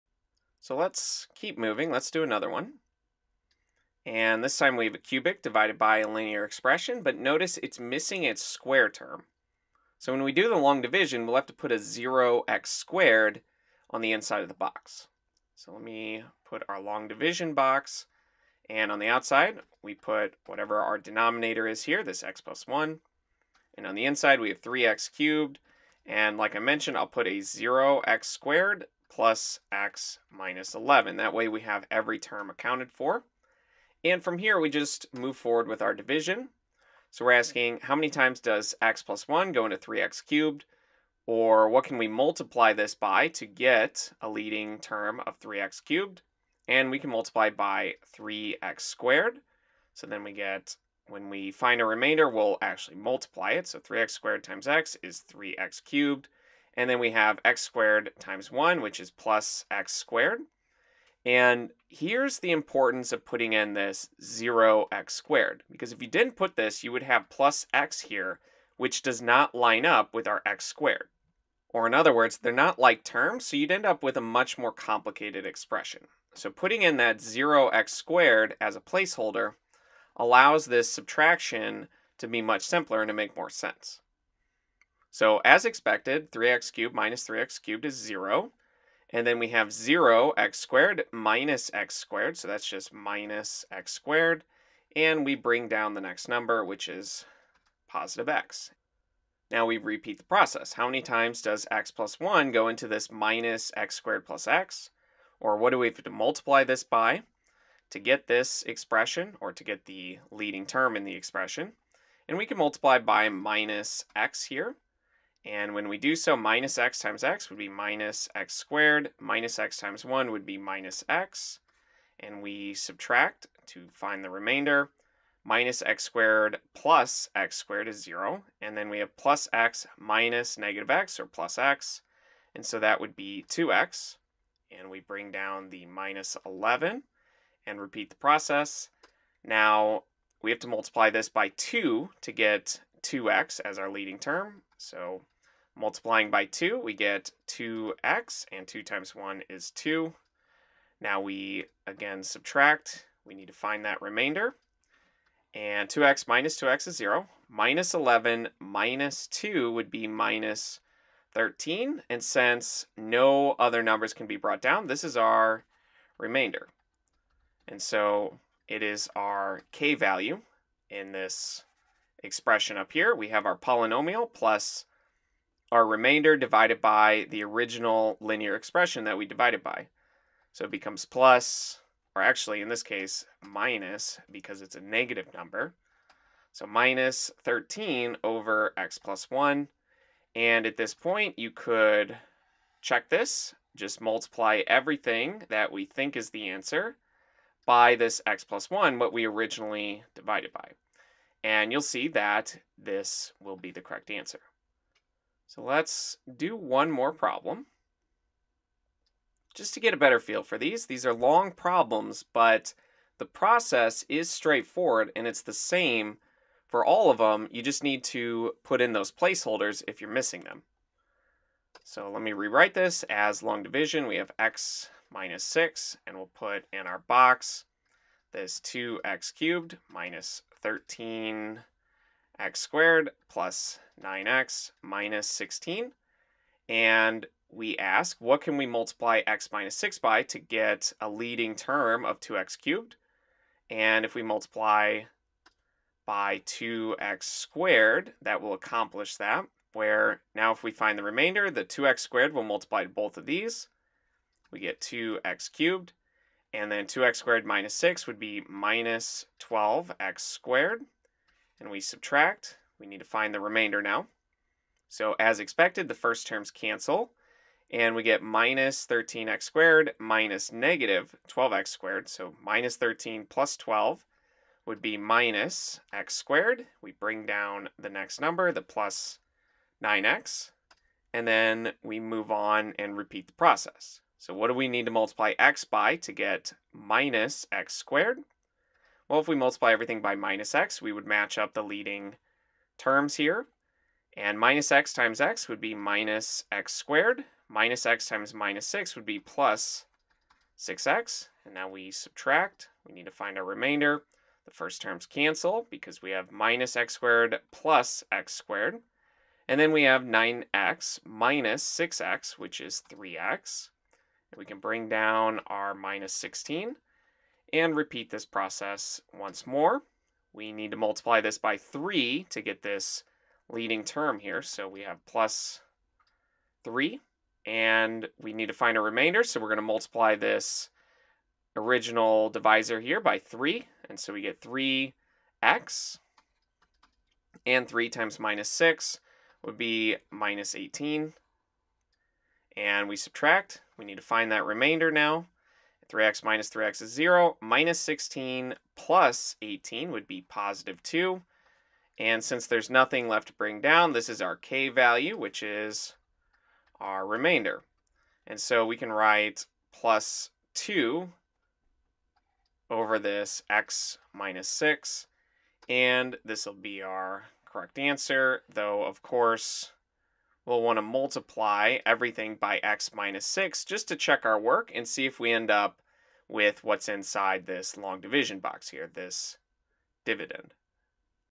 Polynomial Long Division: Practice
This video continues the idea of dividing polynomials by other polynomials by looking at two more example problems.

The general technique of dividing polynomials is similar to long division with numbers.

Khan Academy exercises to practice:
1. Divide Quadratics by Linear Expressions (With Remainders)

2. Divide Polynomials by Linear Expressions

EulersAcademy.org